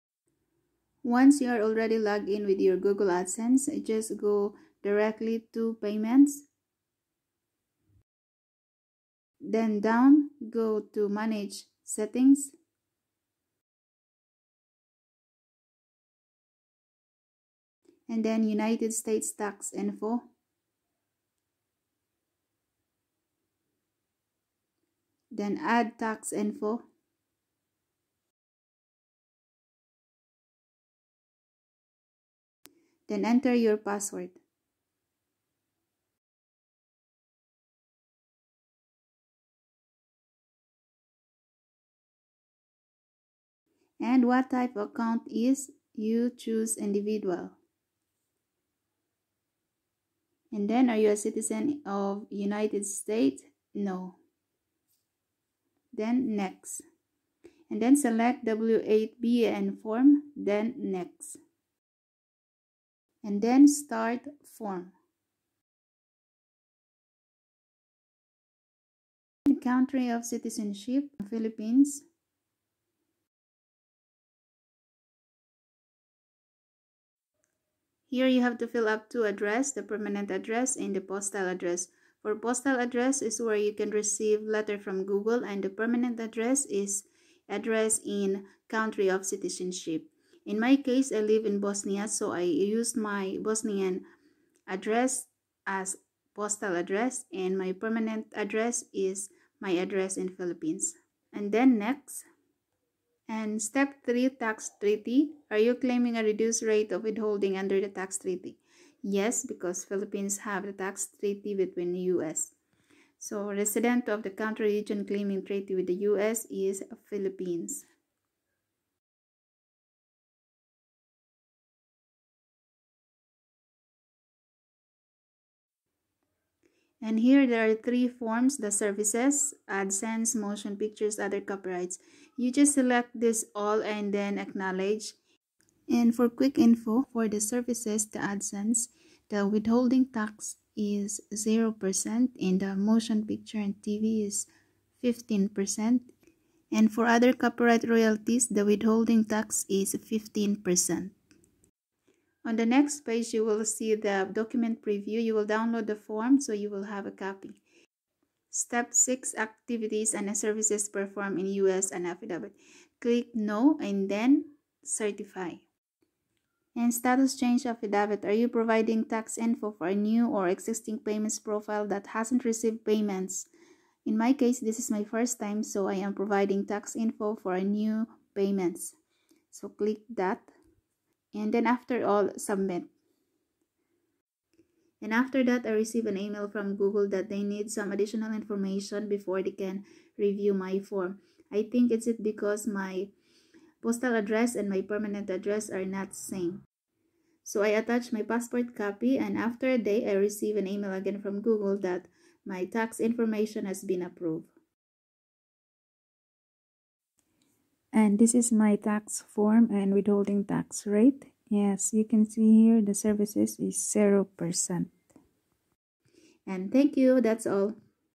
How to update tax info if you are residing abroad|Quick guidelPaano mag update ng tax info abroad
A quick guide on how to update google adsense tax info if you are residing abroad.

Additional documents needed.